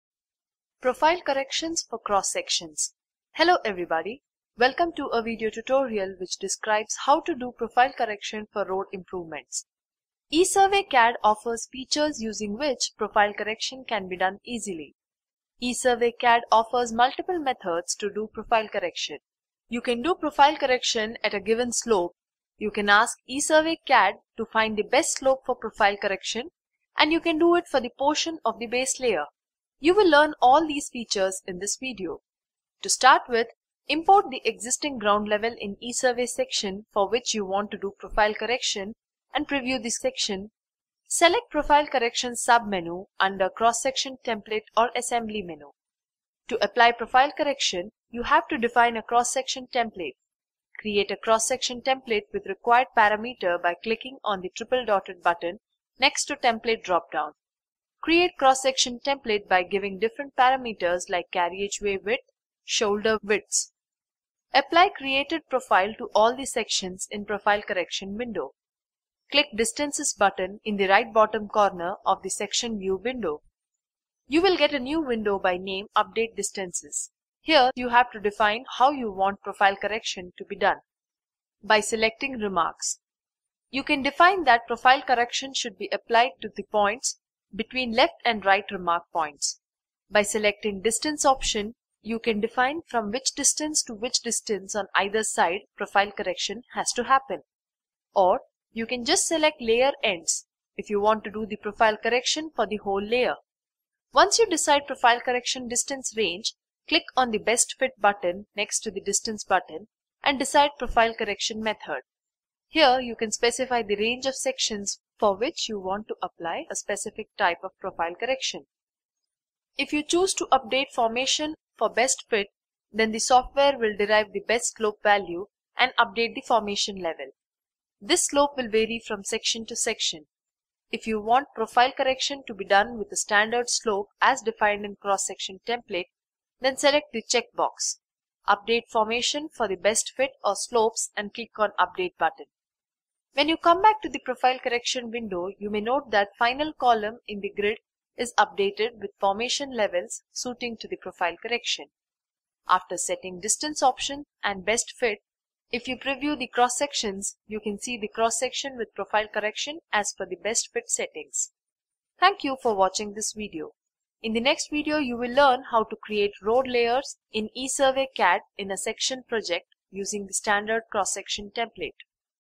Cross Section Profile Correction for Road, Canal, RailRoad Projects
Profile Corrections for different Alignment Projects Such as Road, Canal, Rail Road Projects can be done with Section Module of ESurvey CADD. To apply profile correction you have to define a cross-section template. By setting the required parameter, you can do Profile Correction for all the cross-sections along the whole alignment of the project.

Hello everybody, welcome to a video, tutorial which describes how to do profile correction for road improvements.

ESurvey CADD offers features using which Profile Correction can be done easily. ESurvey CADD offers multiple methods to do profile correction. You can do profile correction at a given slope, you can ask ESurvey CADD to find the best slope for profile correction and you can do it for the portion of the base layer. You will learn all these features in this video.
To start with import the Existing ground level in ESurvey Section for which you want to do profile correction and preview the section.

Select Profile Correction sub-menu under CS Template/Assembly menu
To apply profile correction you have to define a cross-section template. Create a cross-section template with the required parameter by clicking on the triple dotted button next to the Template dropdown. Create CS Template by giving different parameters like Carriageway width, Shoulder widths.

Apply created profile to all the Sections in the profile correction window.
Click the distances button in the Right bottom corner of the Section view window. You will get a new window by name update distances. Here you have to define how you want profile correction to be done.

By  Selecting Remarks – You can define that Profile correction should be applied to points between the left and right remark points.

By selecting distance option you can define from which distance to which distance on either side profile correction has to happen Or you can just select Layer Ends if you want to do the profile correction for the whole layer.

Once you decide the profile correction distance range. Click on the ‘Best fit’ button next to the distance button and decide the profile correction method. Here you can specify the range of sections for which you want to apply a specific type of profile correction. If you choose to update formation for best fit, then the software will derive the best slope value and update the formation level. This slope will vary from section to section. If you want profile correction to be done with a standard slope as defined in CS Template, then select the checkbox, Update formation for best fit / Slopes and click on the update button.

When you come back to the Profile correction window you may note that the final column in the grid is updated with formation levels suiting to profile correction.

After setting distance options and Best Fit. If you preview the Cross-Sections, you can see the cross-section with Profile correction as per Best fit settings.
In the next video, you will learn how to create Road Layers in ESurvey CADD in a section project using the standard CS template.